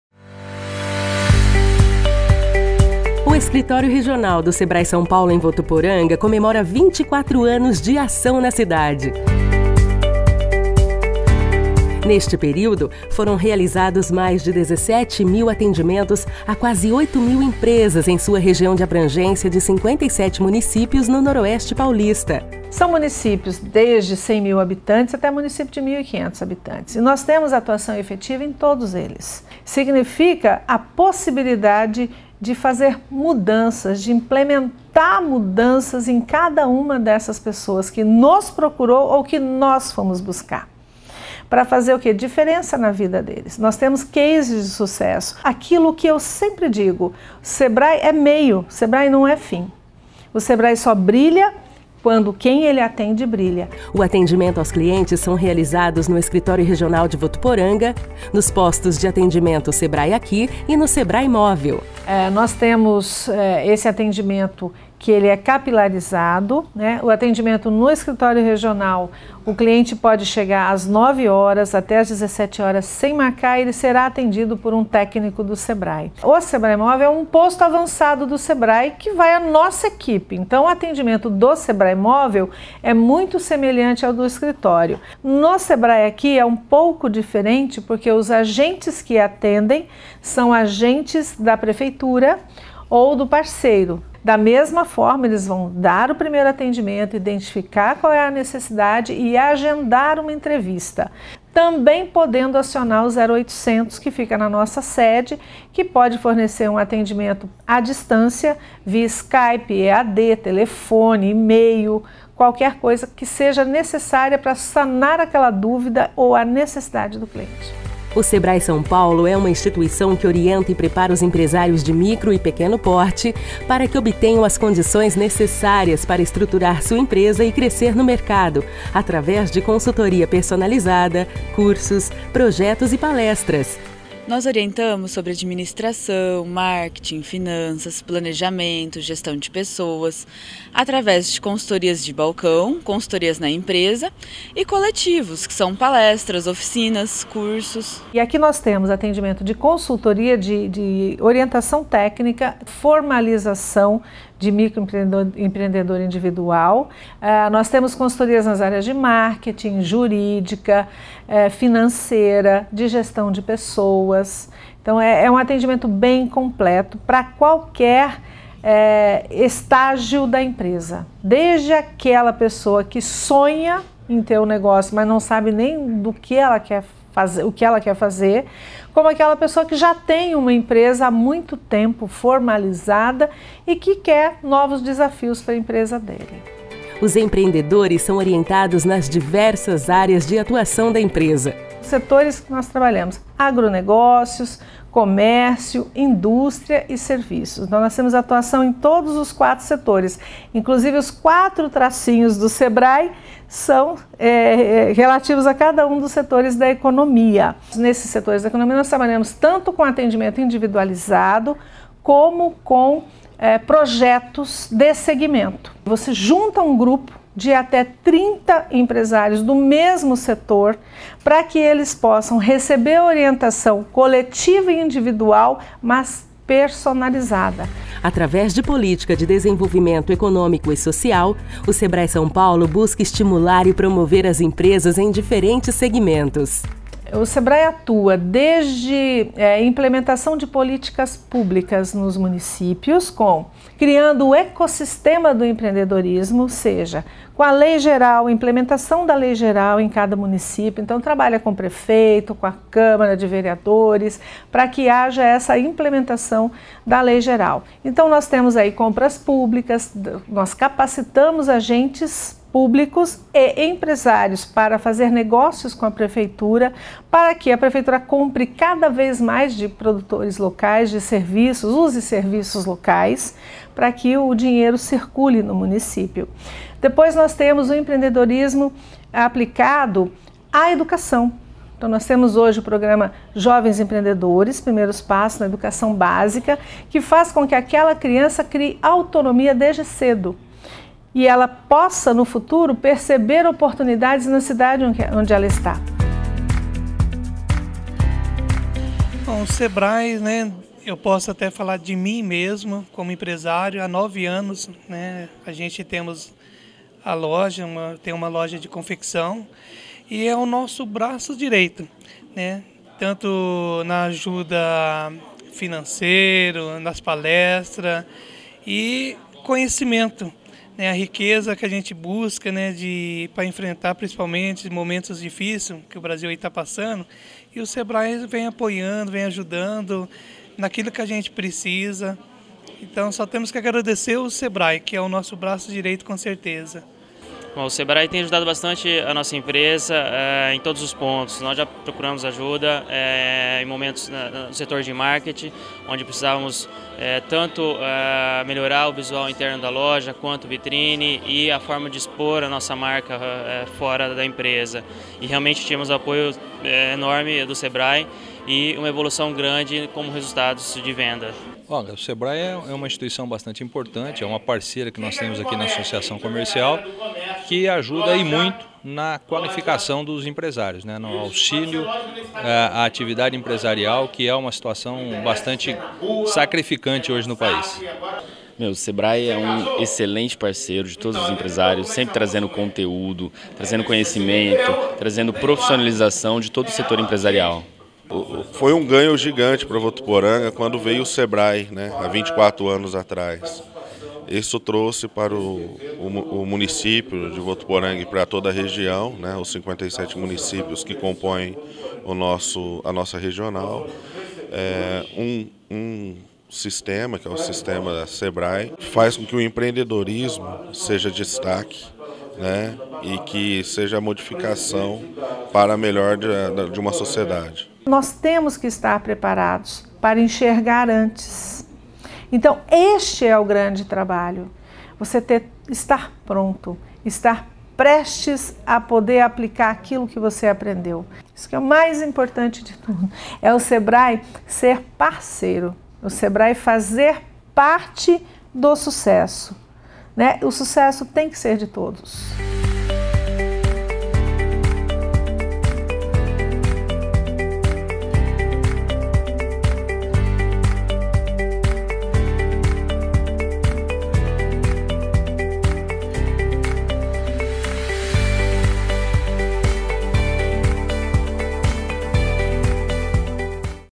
Sebrae-SP está há 24 anos fazendo a diferença em Votuporanga
O Sebrae-SP está há 24 anos implementando mudanças no micro e pequeno negócio de Votuporanga, através de cursos e consultorias feitos no Escritório Regional e atendimentos realizados nos postos do Sebrae Aqui e o Sebrae Móvel. Confira os depoimentos no vídeo, falando mais sobre como o Sebrae-SP atua na região.